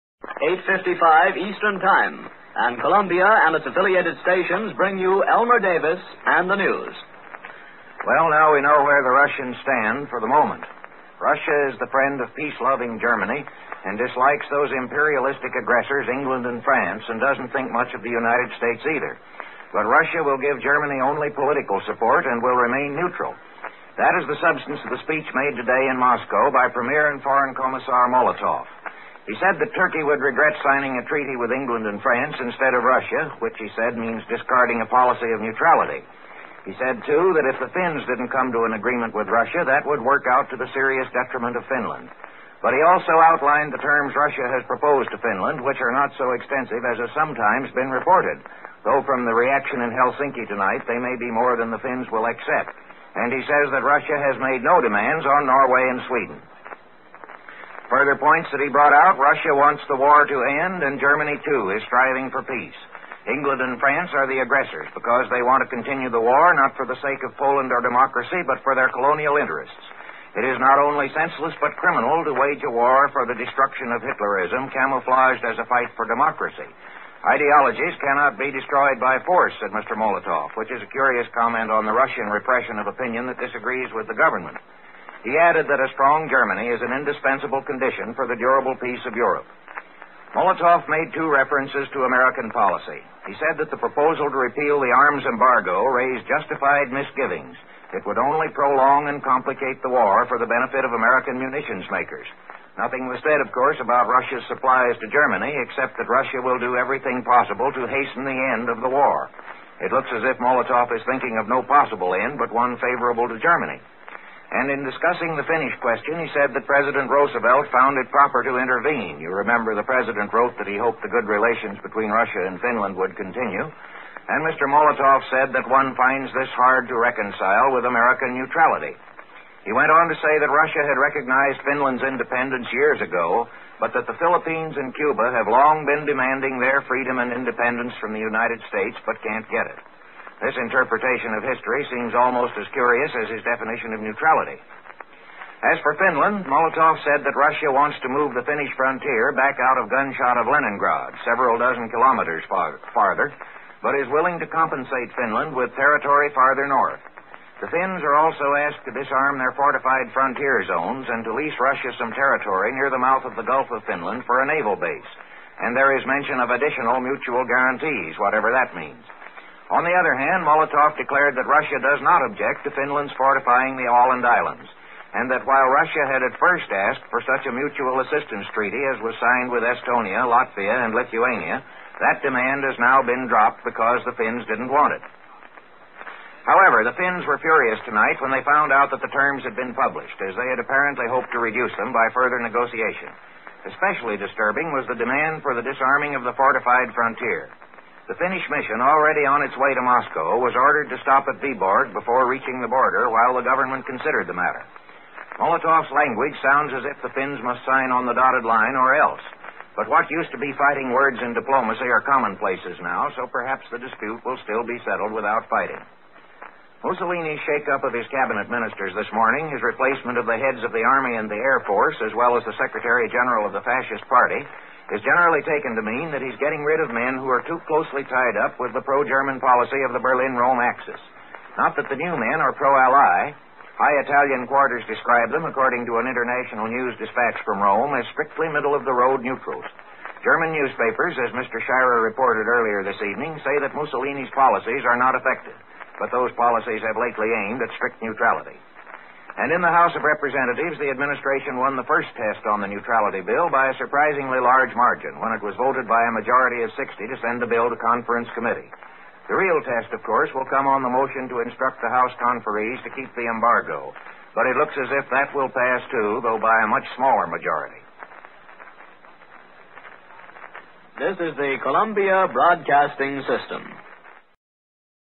CBS - Elmer Davis Reports On Molotovs Speech - October 31, 1939
Audio and Image are Public Domain (Copyright, Designs and Patents Act 1988)